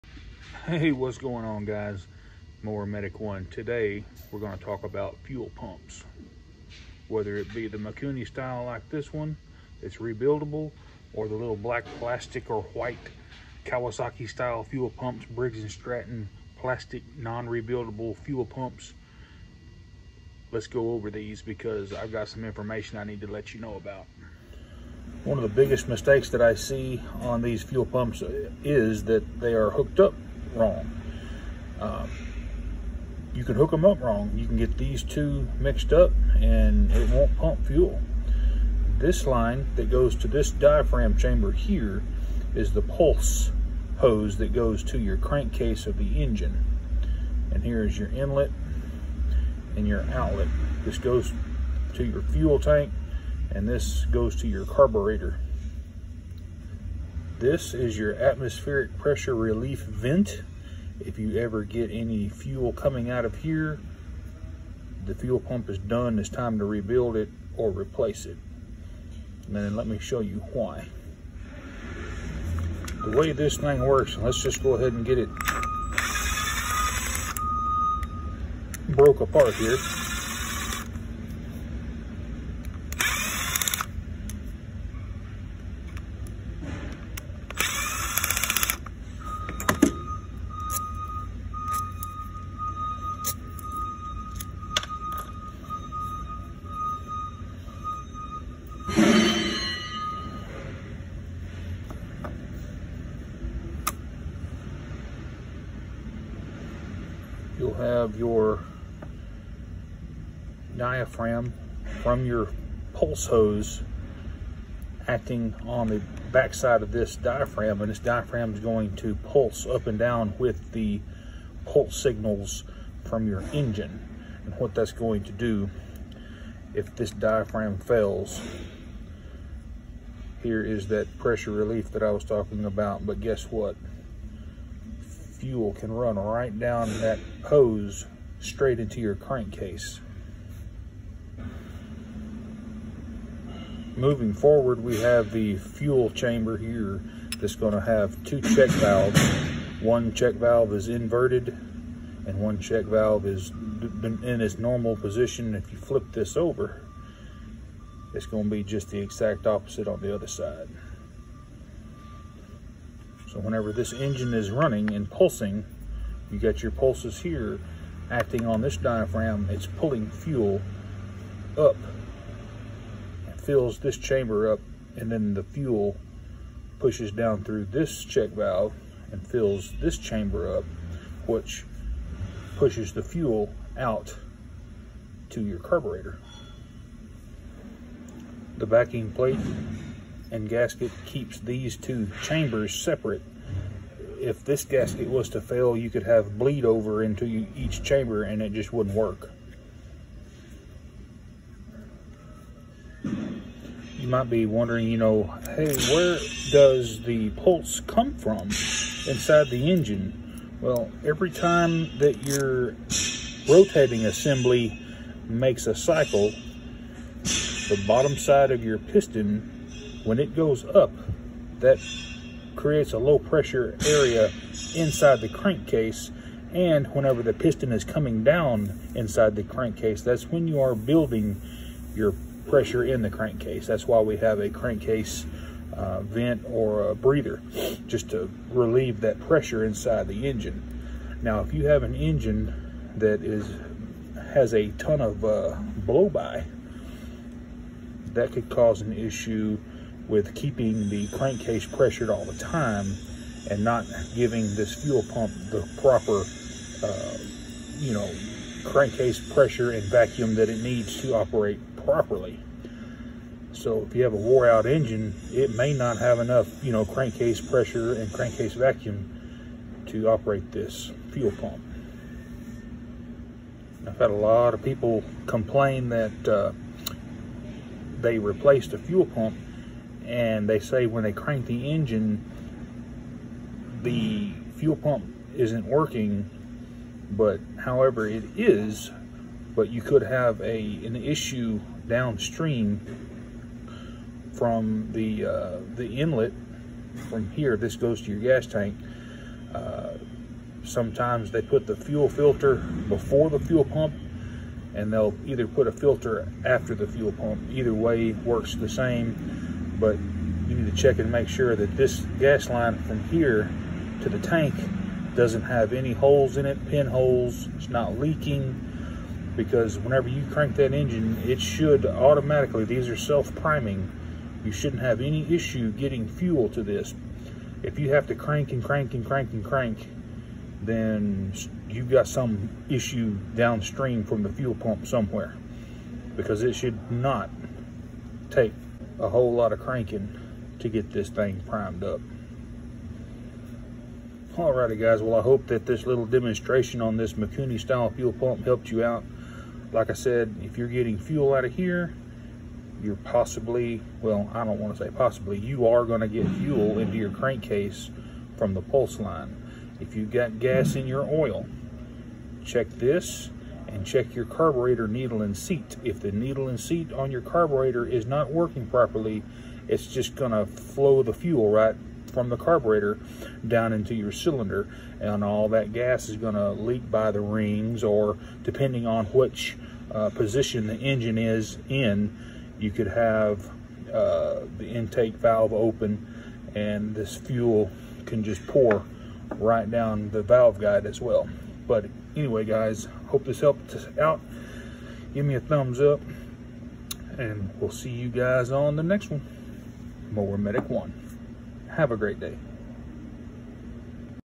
FUELPUMPS / WHAT FAILS? / GAS IN OIL? / THEN YOU NEED TO GO CHECK THIS RIGHT NOW!
Today we take an in-depth look on pulse style fuel pumps, Whether it be the Briggs & Stratton or Kawasaki style fuel pump or the rebuildable Mikuni style fuel pump shown in my video, let’s go over the operation And failure points of most diaphragm style fuel pumps